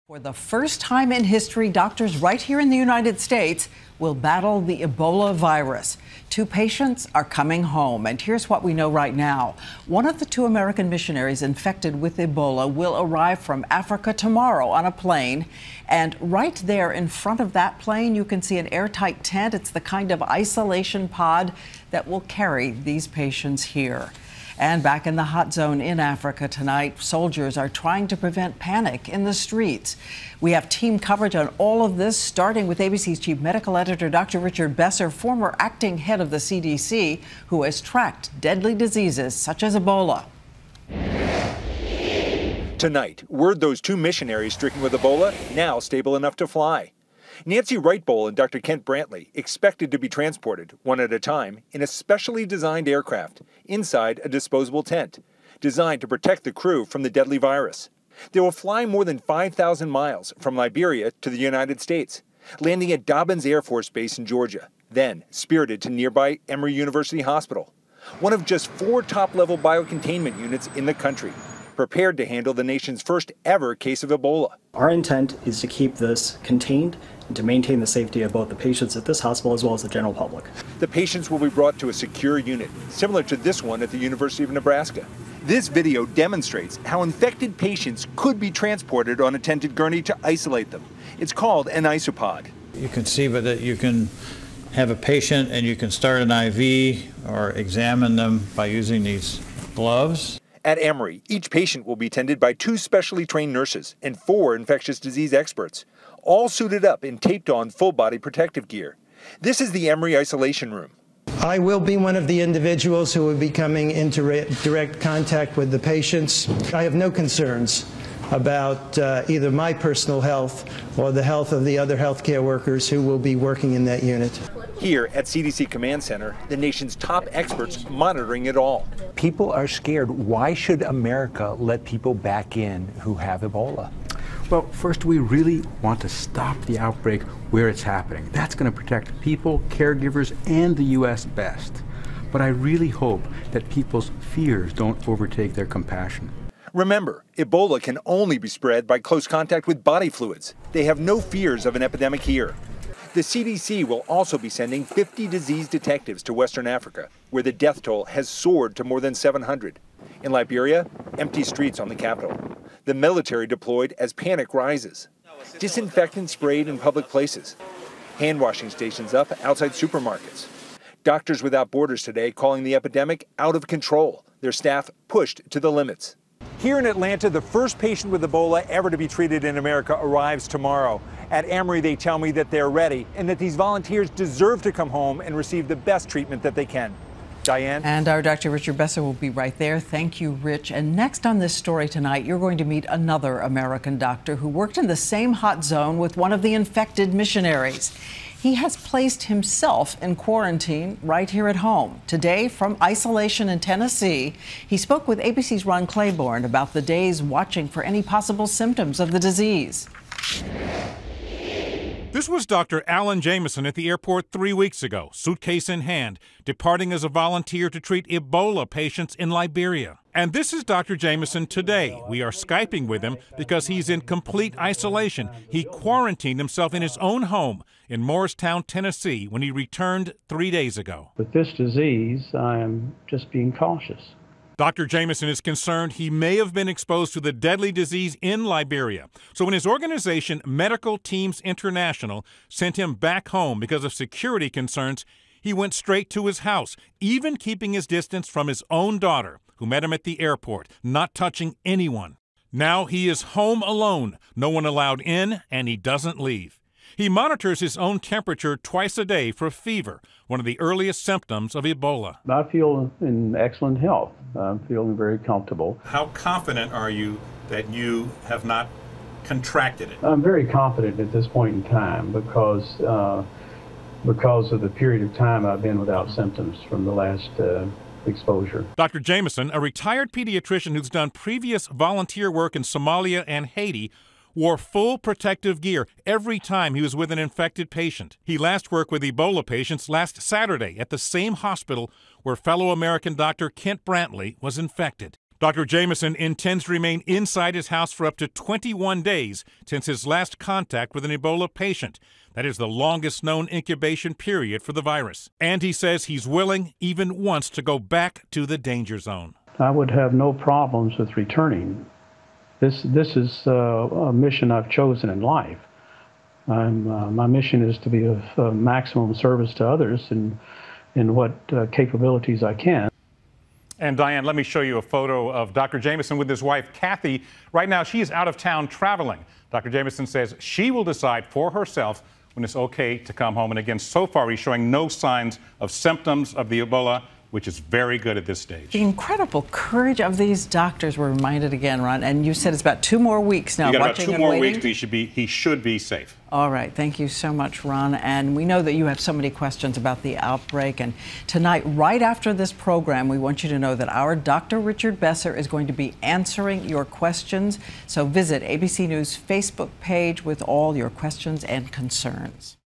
Battling the Ebola Virus at Home
Two patients stricken with the virus are coming home to the US and one more US doctor has placed himself in quarantine.